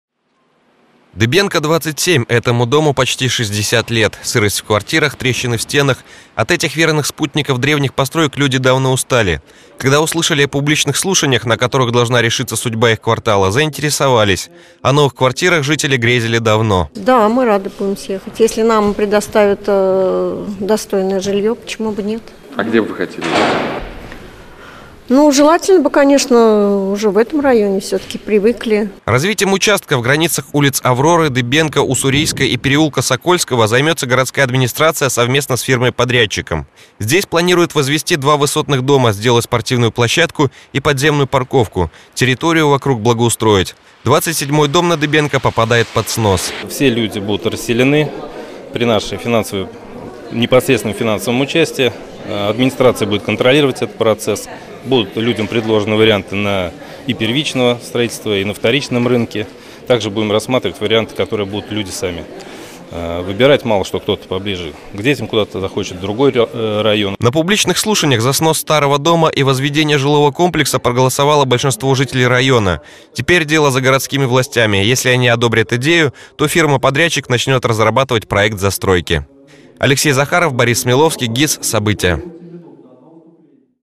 Два новых дома вместо одного старого
В администрации Советского района прошли публичные слушания. Тема - судьба участка в границах улиц Авроры, Дыбенко, Уссурийской и переулка Сокольского. Здесь собираются сносить ветхое жилье и возводить жилой комплекс. Что по этому поводу думают жители района - в нашем сюжете.